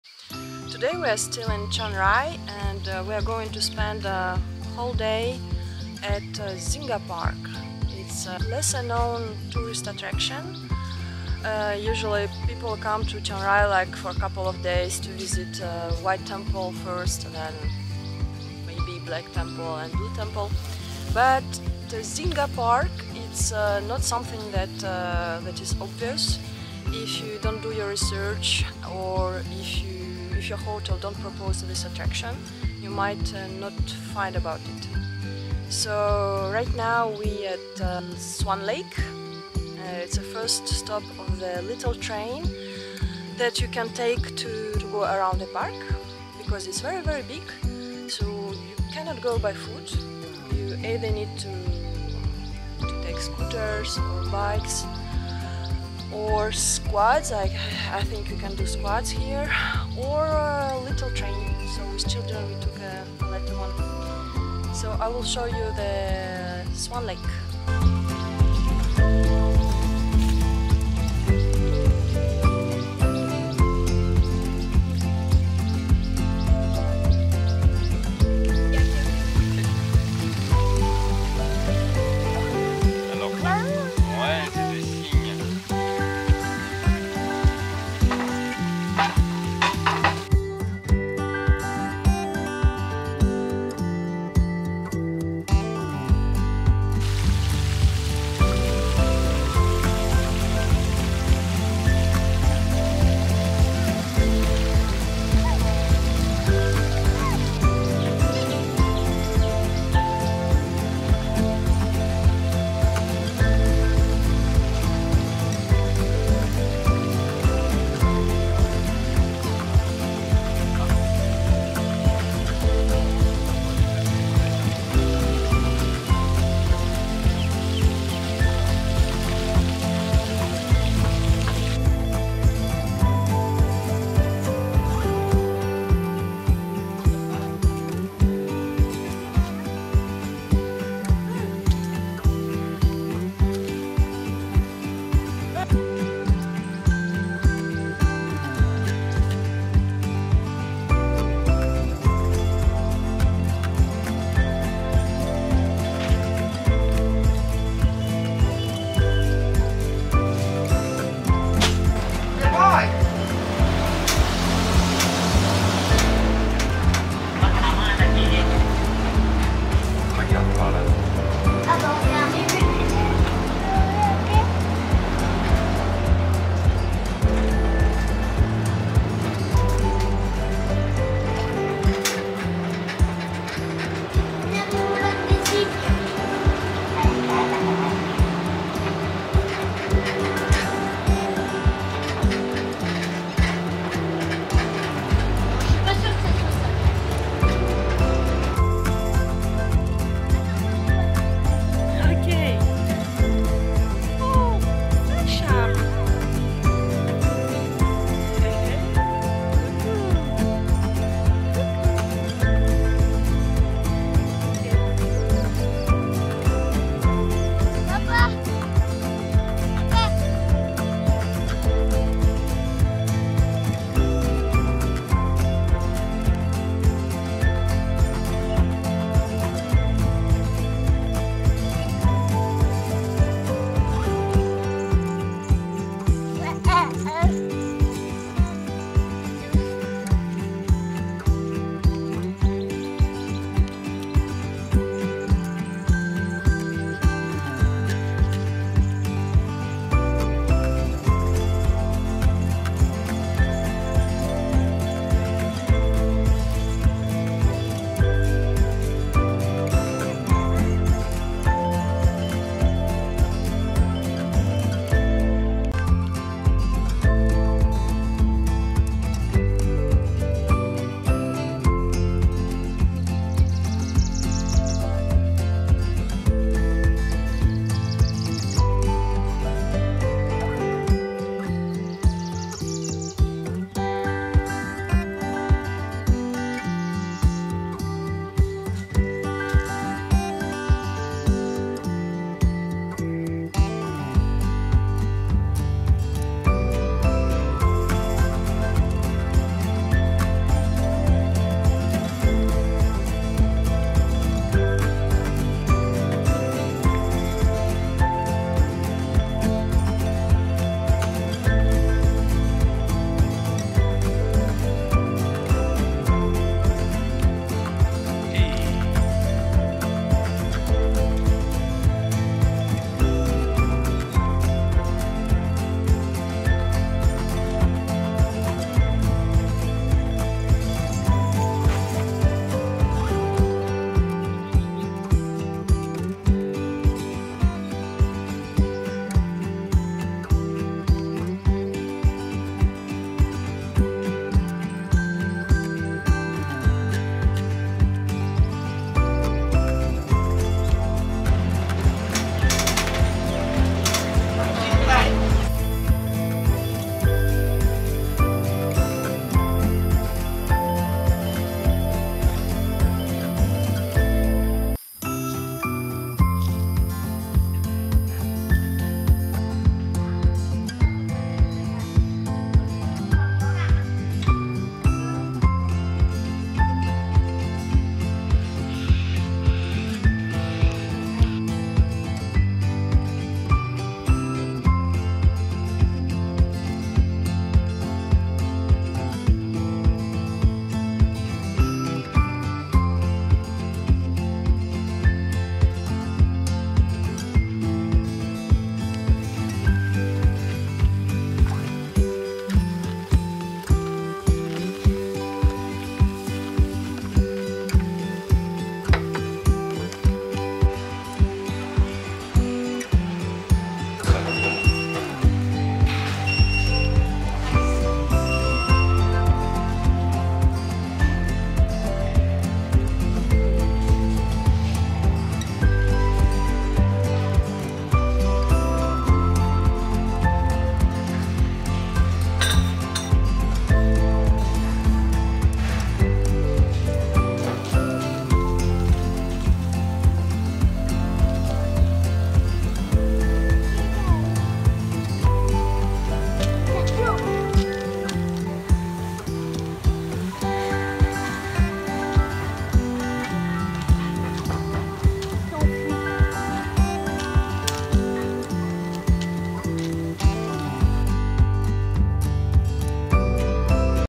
Perfect Day With Kids in Singha Park (Chiang Rai)
Place where you can easily spend a day or two with kids in Chiang Rai